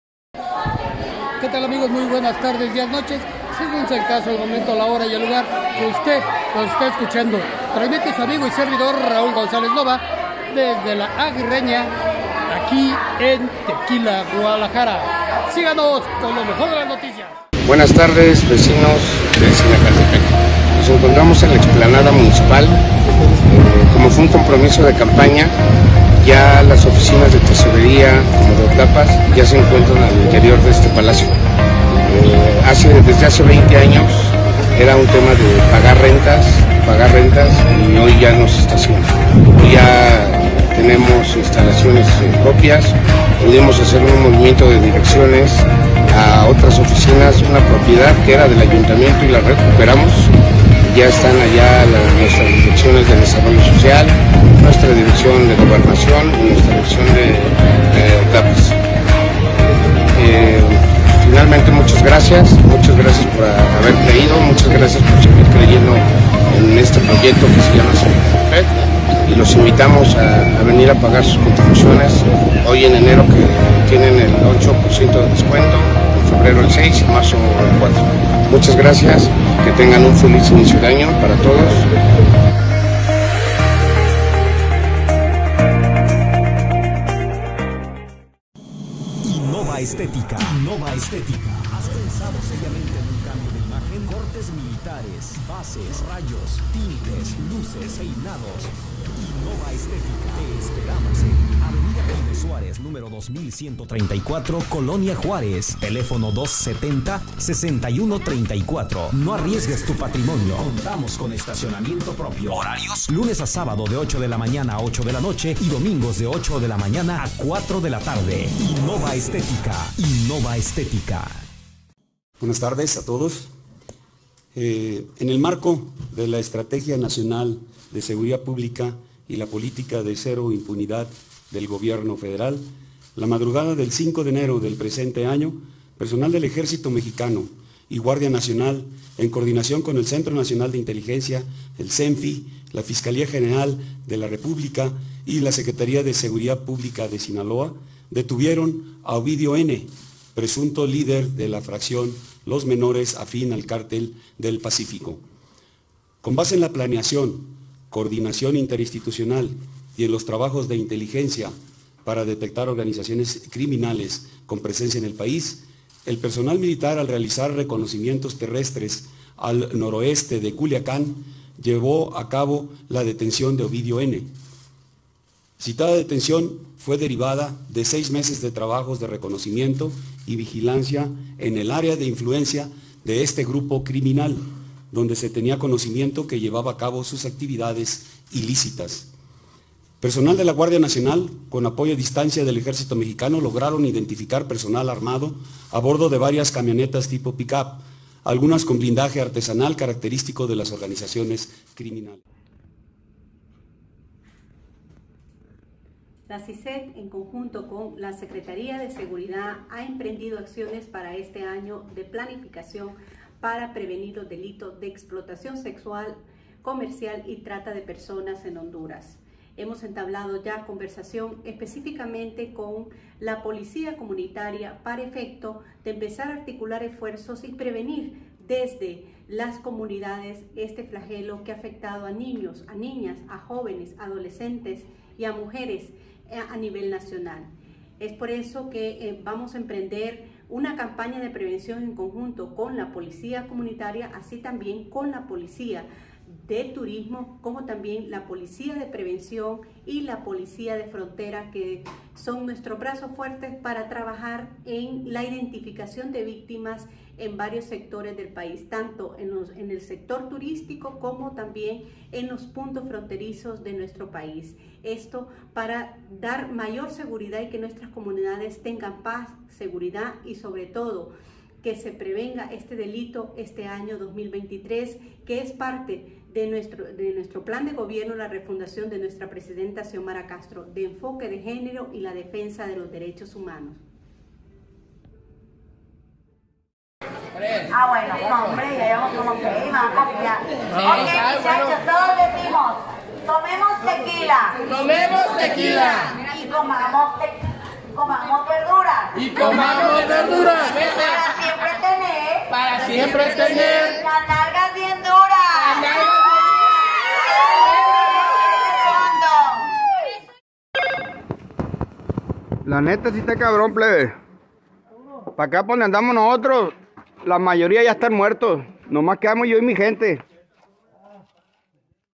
* Manuel Vilchis alcalde de Zinacantepec* InnovaEstetica* Estrategia Nacional de Seguridad Públic
* Manuel Vilchis alcalde de Zinacantepec
* Estrategia Nacional de Seguridad Pública y Cero Impunidad
* Secretaria de seguridad previene los delitos sexuales y trata de personas en Honduras
*  Los dichos de Jalisco en Tequila
* Lochusco (la neta si esta cabron)